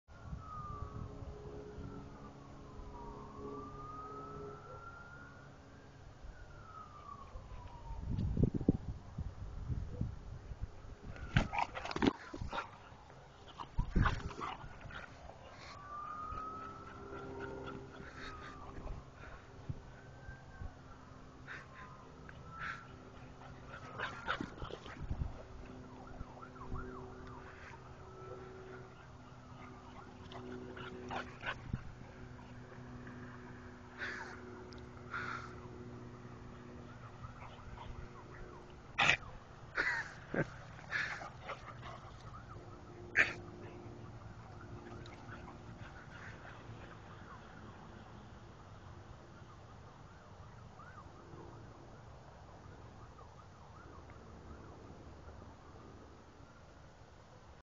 French Bulldog takes out a German Shorthair
French bulldog undercuts shorthair